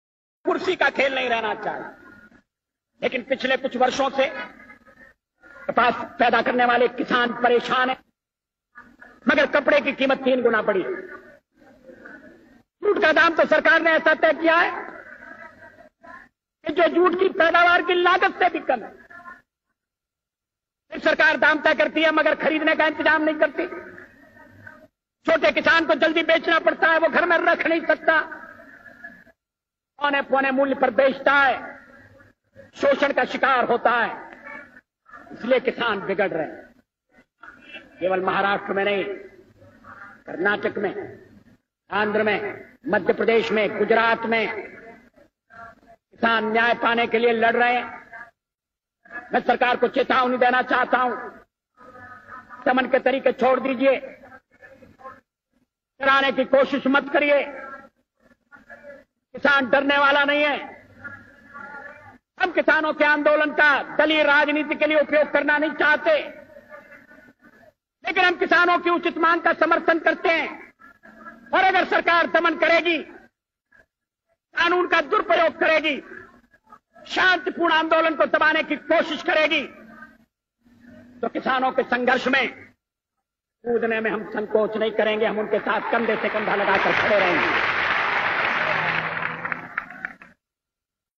Atal Biahari Bajpeyi old speech... Kishan Aandolan by Atal bihari bajpeyi..atal kavita
90 के दशक में किसान आंदोलन पर अटल बिहारी वाजपेयी जी का सरकार के खिलाफ भाषण..

Atal bihari bajpeyi ji kavita kavi sammelan, kisan aandolan speech by atal ji..

कपास के किसानों के लिए आंदोलन..
अटल बिहारी बाजपेयी जी का महाराष्ट्र में आंदोलन..